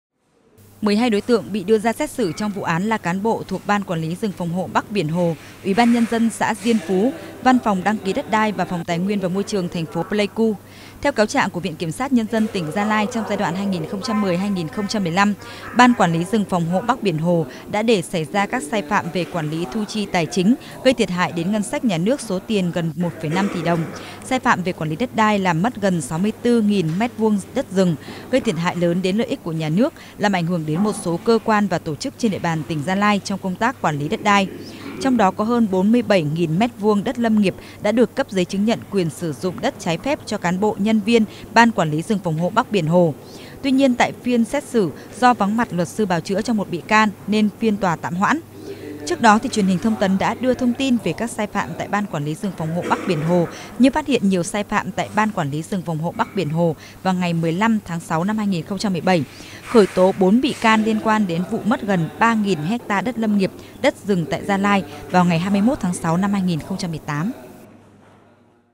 Hoãn phiên xét xử vụ sai phạm tại Ban quản lý rừng phòng hộ Bắc Biển Hồ
Sáng 3/10, Tòa án nhân dân tỉnh Gia Lai mở phiên xét xử sơ thẩm 12 bị can liên quan đến hàng loạt sai phạm tại Ban quản lý rừng phòng hộ Bắc Biển Hồ.